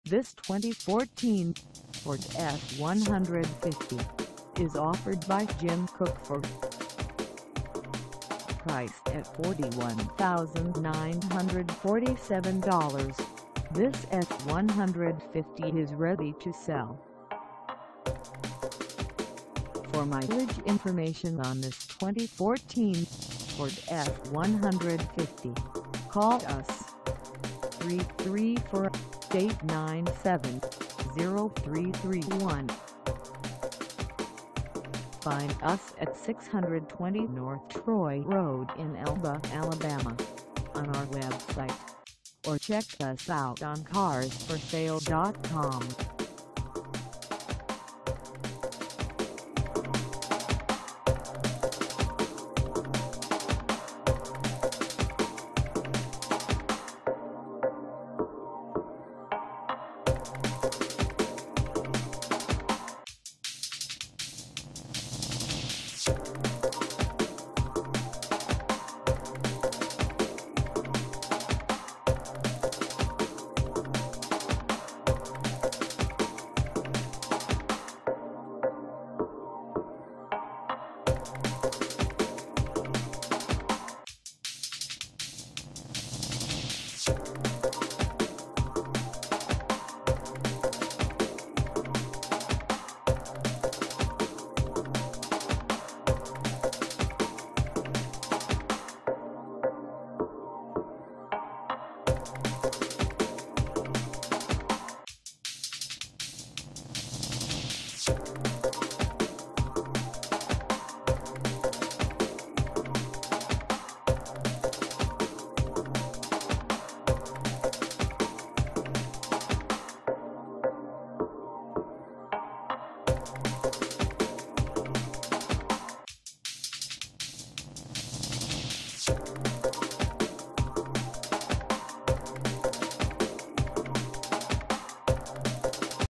2014 Ford F-150 XLT for sale in ELBA, AL 36323 at the Jim Co
This 2014 Ford F-150 XLT is for sale in ELBA, AL 36323 at Jim Cook Ford.
Engine: V6 3.5L V6
Transmission: Automatic 6-Speed
Color: Kodiak Brown Metallic
Mileage: 0
Contact Jim Cook Ford 620 N Troy Road ELBA, AL 36323 to test drive this 2014 Ford F-150 XLT today.